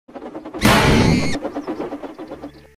(P3D )FNAF UNIVERSES) A test
as you can see I have one of the models.(not really it's not just one...)  from in heat I almost have some of the girls I recently extracted the girls from in heat and ported in Prisma 3d cuz why not  thinking of releasing them soon who knows it could happen or which will probably most likely happen

obviously anyways see you which will probably be in an hour or most likely you'll know when I upload I don't know why I'm telling you this sister I'll see you whenever I guess.